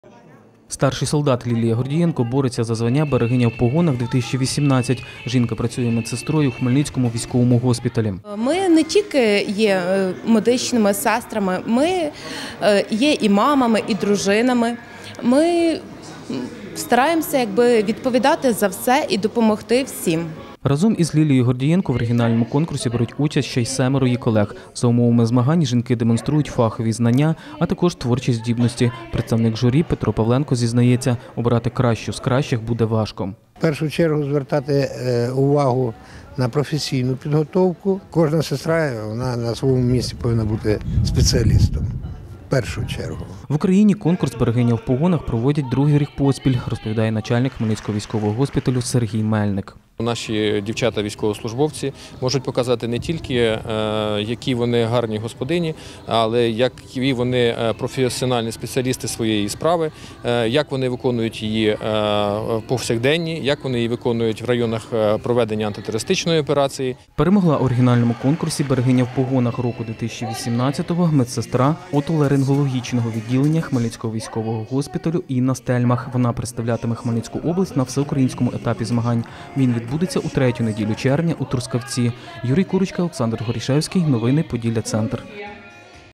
«Берегиню в погонах 2018» обрали серед жінок медиків Хмельницького військового шпиталю
Конкурс серед жінок-військовослужбовців медичної служби  відбувся нині у Хмельницькому15 гарнізонному будинку офіцерів. За звання «Берегиня в погонах 2018» боролися вісім учасниць. Як розповів начальник Хмельницького військового госпіталю Сергій Мельник, це регіональний етап змагань. Їхня переможниця візьме участь у Всеармійському конкурсі, який відбудеться  у Трускавці.